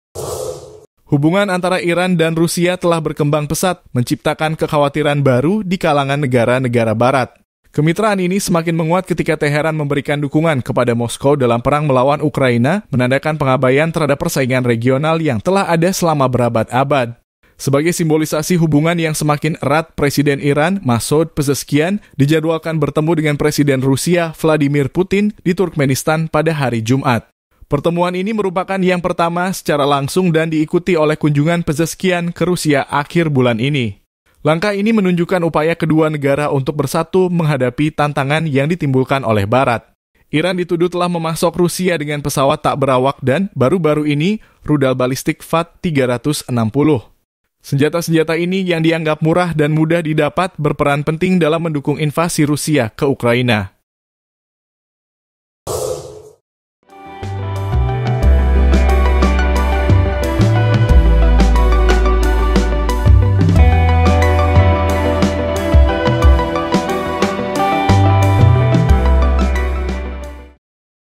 Meningkatnya Hubungan Antara Iran dan Rusia Membuat Gentar Ameriak dan Negara Barat
Hubungan antara Iran dan Rusia telah berkembang pesat, menciptakan kekhawatiran baru di kalangan negara-negara Barat.

Kemitraan ini semakin menguat ketika Teheran memberikan dukungan kepada Moskow dalam perang melawan Ukraina, menandakan pengabaian terhadap persaingan regional yang telah ada selama berabad-abad.

Sebagai simbolisasi hubungan yang semakin erat, Presiden Iran, Masoud Pezeshkian, dijadwalkan bertemu dengan Presiden Rusia, Vladimir Putin, di Turkmenistan pada hari Jumat.